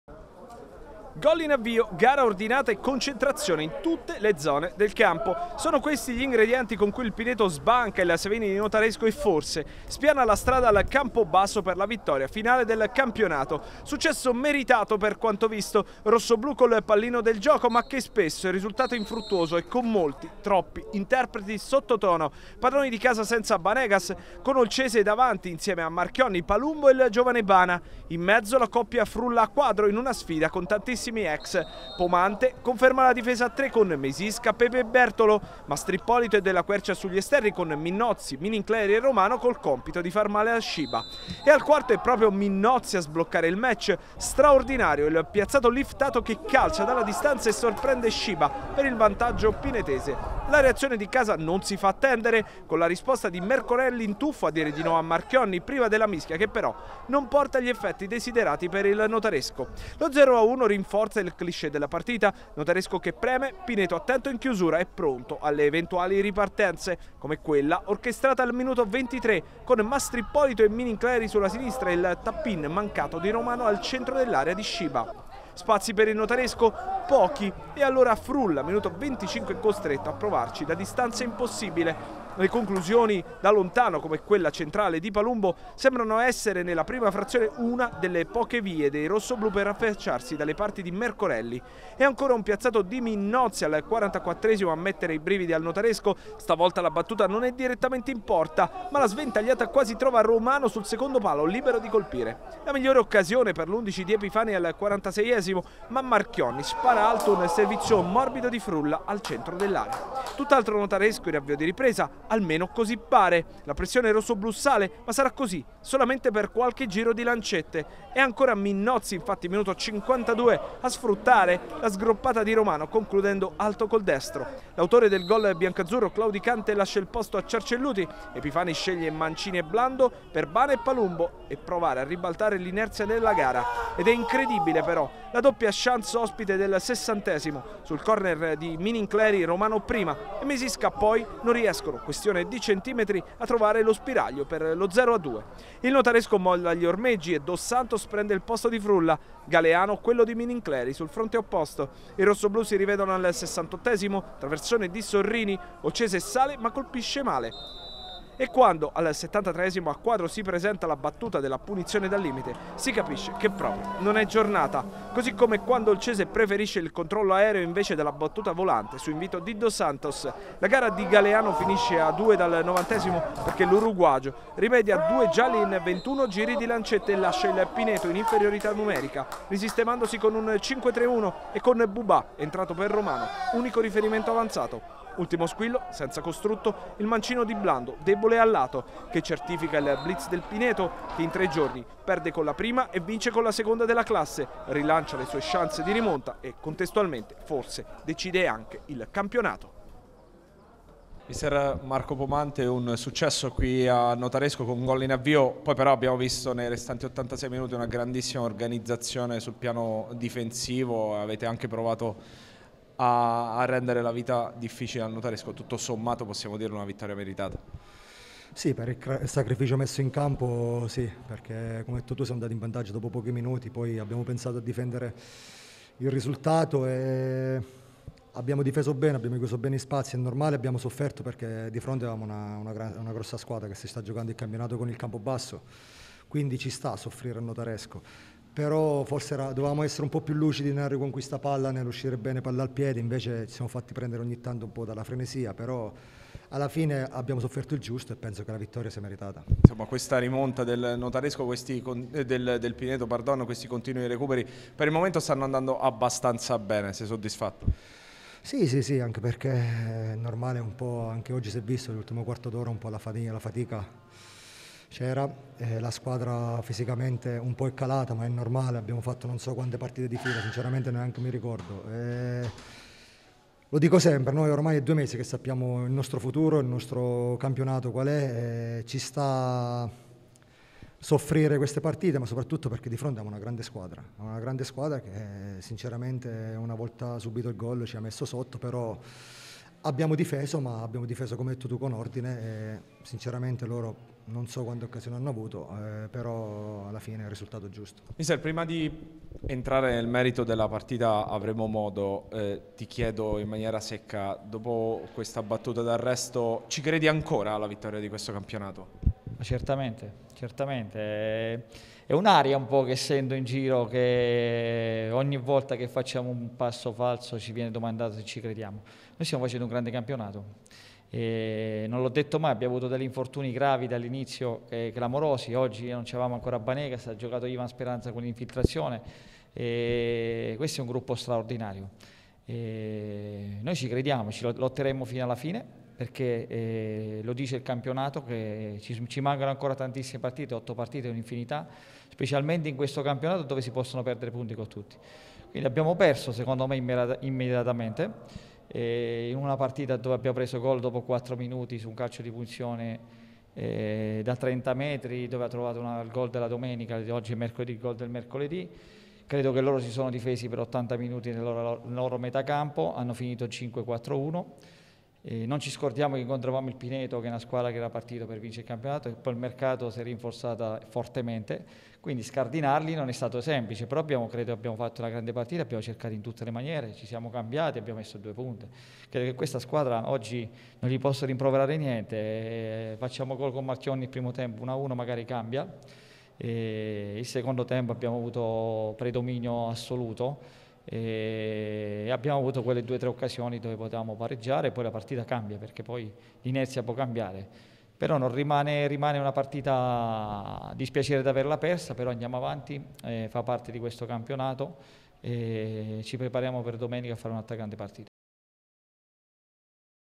Serie D, il derby va al Pineto. Cade il Notaresco (0-1)
Nel pomeriggio in Serie D recupero della prima giornata di ritorno del Girone F con il successo del Pineto sul campo del Notaresco. La rete di Minnozzi in apertura regala i tre punti alla formazione di Marco Pomante e costringe i rossoblu a rimanere a -5 dal Campobasso.